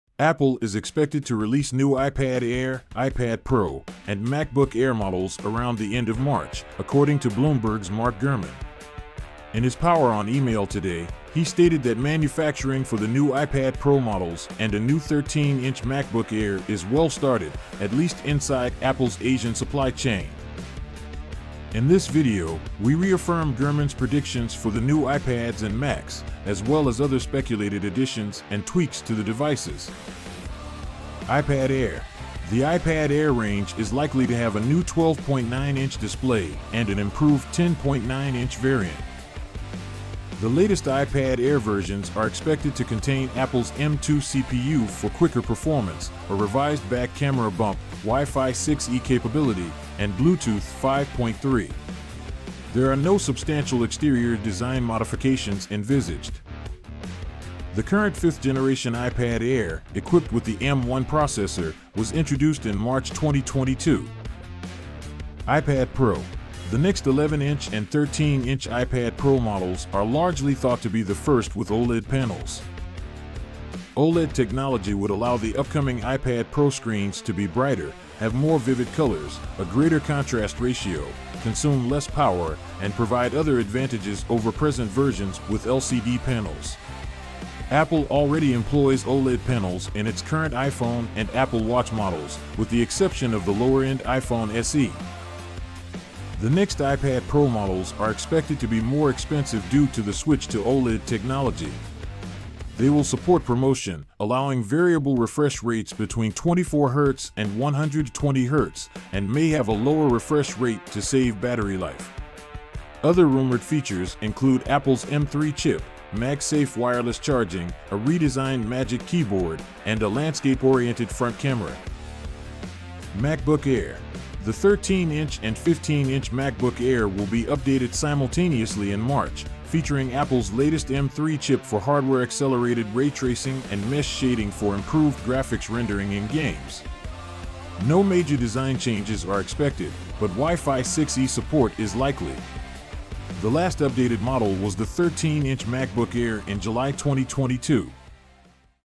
New iPads and Macs Likely to Launch in Late March
Apple is expected to release new iPad Air, iPad Pro, and MacBook Air models "around the end of March," according to Bloomberg's Mark Gurman. In his Power On email today, he stated that manufacturing for the new iPad Pro models and a new 13-inch MacBook Air is well started, at least inside Apple's Asian supply chain.

In this video, we reaffirm Gurman's predictions for the new iPads and Macs, as well as other speculated additions and tweaks to the devices.

iPad Air

The iPad Air range is likely to have a new 12.9-inch display and an improved 10.9-inch variant.

The latest iPad Air versions are expected to contain Apple's M2 CPU for quicker performance, a revised back camera bump, Wi-Fi 6E capability, and Bluetooth 5.3. There are no substantial exterior design modifications envisaged.

The current fifth-generation iPad Air, equipped with the M1 processor, was introduced in March 2022.

iPad Pro

The next 11-inch and 13-inch iPad Pro models are largely thought to be the first with OLED panels.

OLED technology would allow the upcoming iPad Pro screens to be brighter, have more vivid colors, a greater contrast ratio, consume less power, and provide other advantages over present versions with LCD panels. Apple already employs OLED panels in its current iPhone and Apple Watch models, with the exception of the lower-end iPhone SE.

The next iPad Pro models are expected to be more expensive due to the switch to OLED technology. They will support ProMotion, allowing variable refresh rates between 24Hz and 120Hz, and may have a lower refresh rate to save battery life. Other rumored features include Apple's M3 chip, MagSafe wireless charging, a redesigned Magic Keyboard, and a landscape-oriented front camera.

MacBook Air

The 13-inch and 15-inch MacBook Air will be updated simultaneously in March, featuring Apple's latest M3 chip for hardware-accelerated ray tracing and mesh shading for improved graphics rendering in games. No major design changes are expected, but Wi-Fi 6E support is likely. The last updated model was the 13-inch MacBook Air in July 2022.